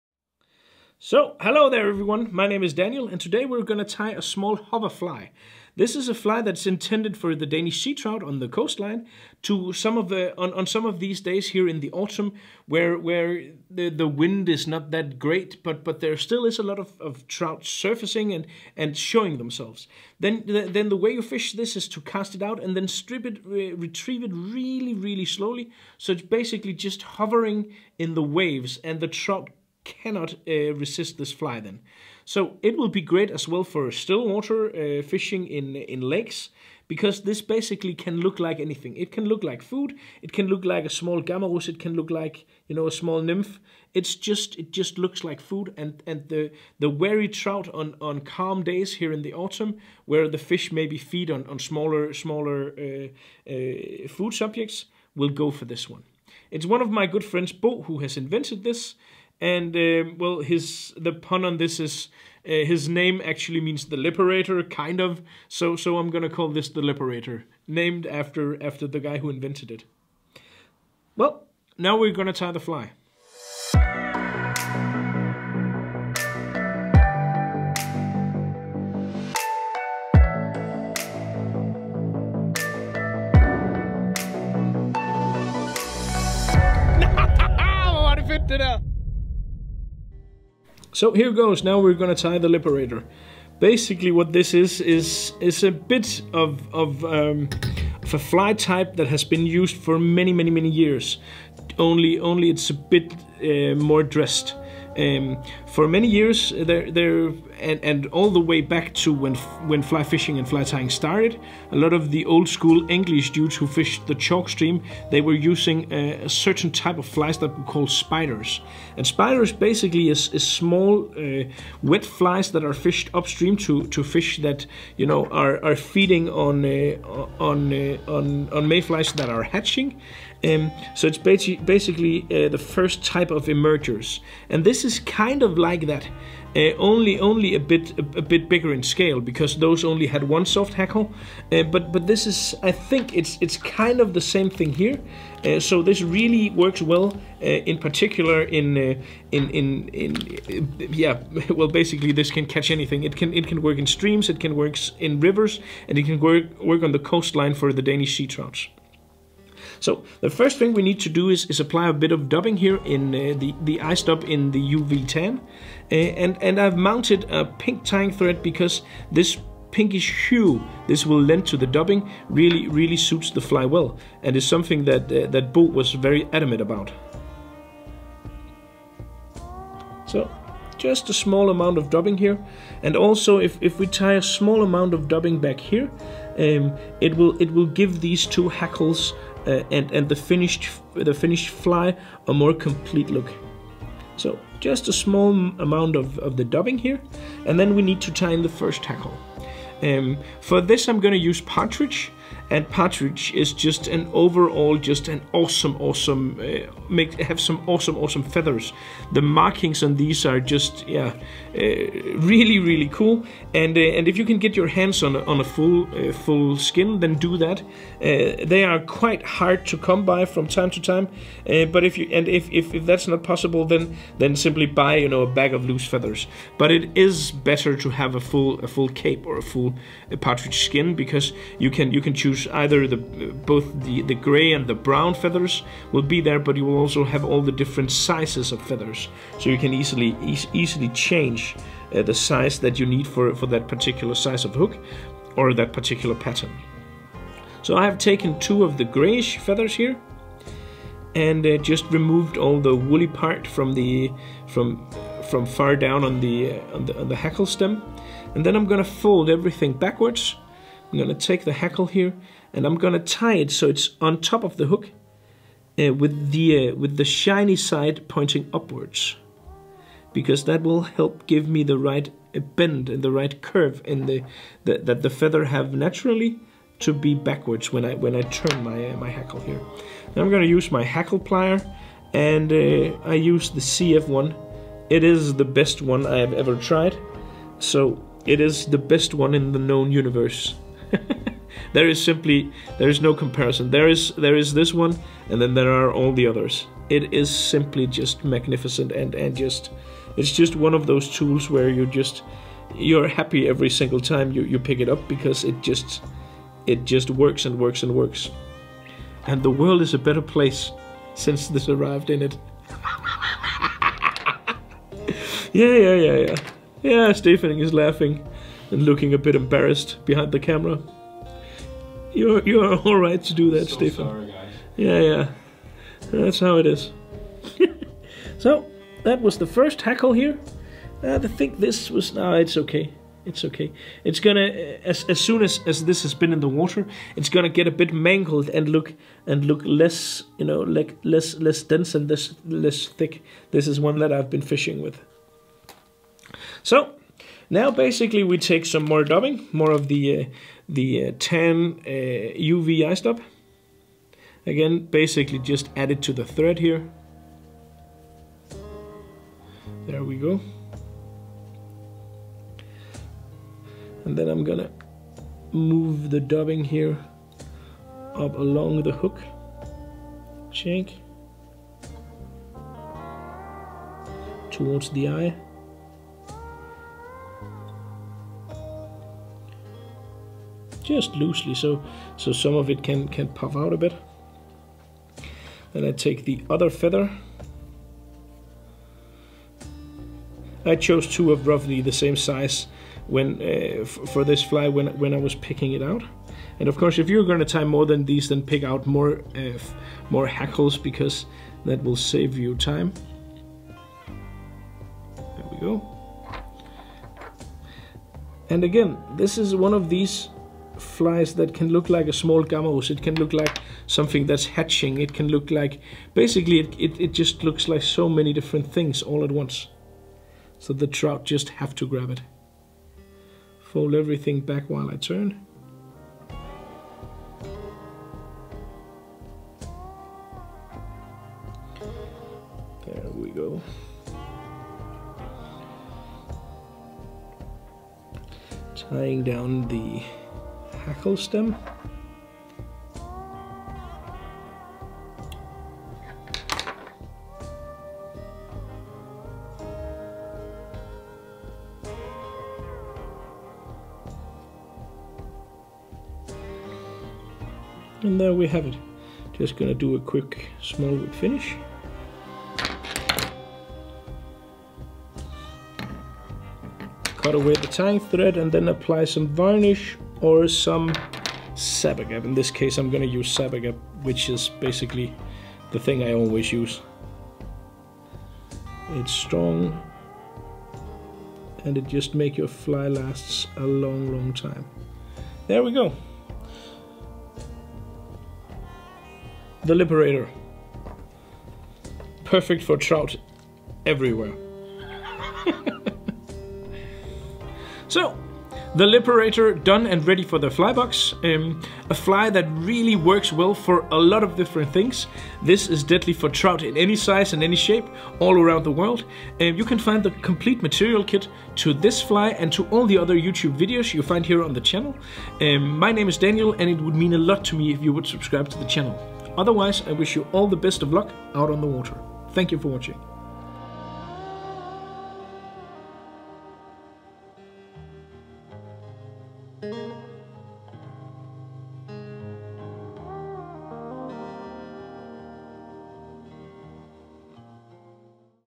THE LIBERATOR - Hoverfly for seatrout
This time around Daniel is tying the liberator. A pattern from Bo Frier, for the picky sea trout in the baltic sea. Genius fly in the autumn, when the fish are extra picky.